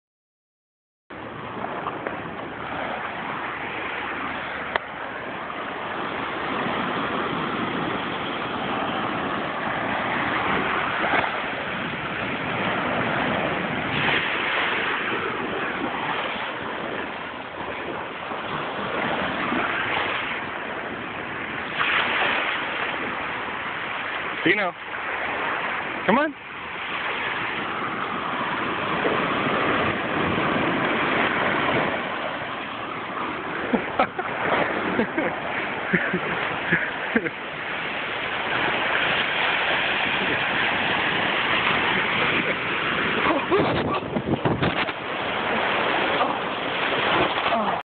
Dino the Airedale Terrorier
Getting tackled by my psycho Airdale puppy Dino